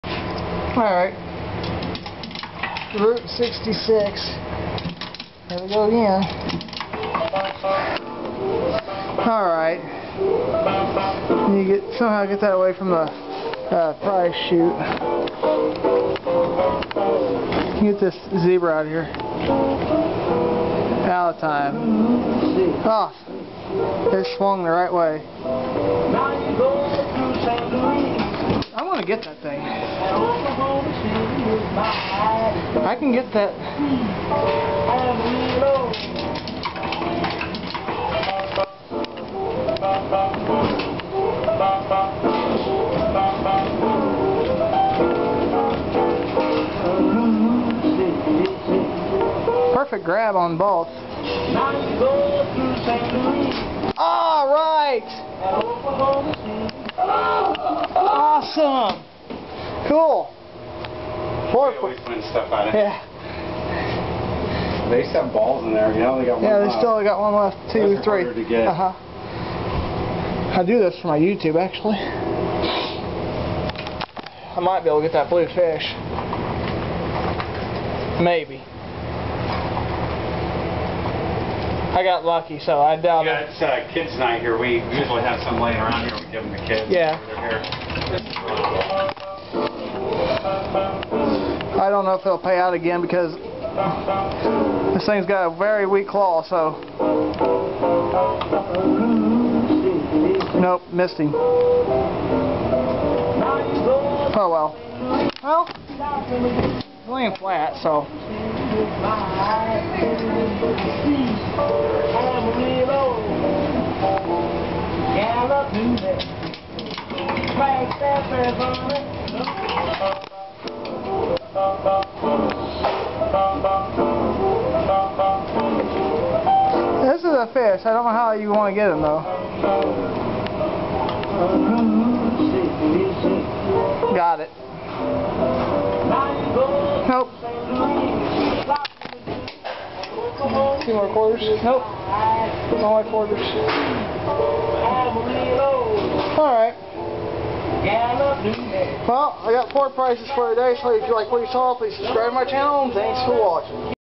ROUTE 66 CLAW MACHINE@ PIZZA HUT A 2 TO 1 WIN
THIS WAS FILMED YESTERDAY AT MY LOCAL PIZZA HUT AND HERE I AM PLAYING THE ROUTE 66 CLAW MACHINE HERE KEEP UR EYE ON THE CLAW AS IT OPENS UP AND LETS GO OF THINGS IF U WOULD LIKE TO CHECK OUT THE VIDEO CALLED ROUTE 66 CLAW MACHINE @PIZZA HUT A HARD MACHINE TO WIN OUT OF LET ME KNOW WHAT U THINK